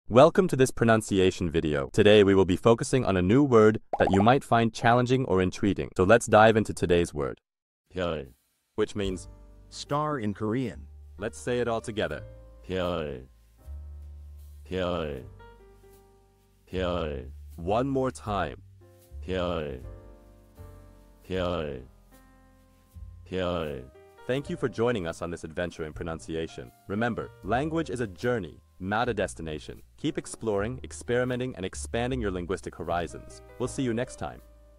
How to pronounce byeol | 별 (star in Korean)
Master the Pronunciation of 'byeol | 별' - which means : star in Korean 📝 with @PronunciationKorean 🎓🗣️ - [Your Guide to Korean]

Learn to pronounce 'byeol' perfectly in Korean with our easy and engaging tutorial by @PronunciationKorean! 📖📽️ In this comprehensive video, we provide you with native speaker examples 🗣️👂 and phonetic breakdowns 🎵 to ensure you get the pronunciation just right.